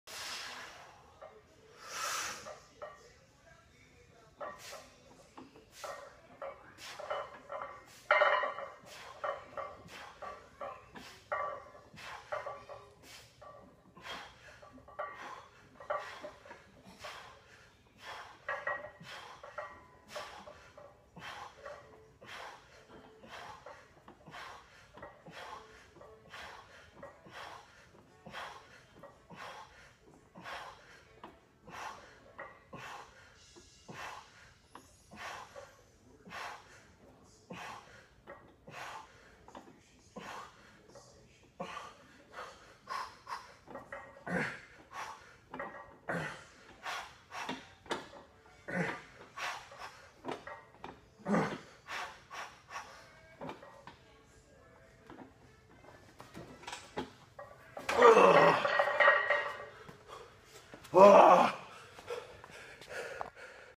225x40 Bench
Beastmode Training BW245 36yr old ULTRABEAST Will Dinwiddie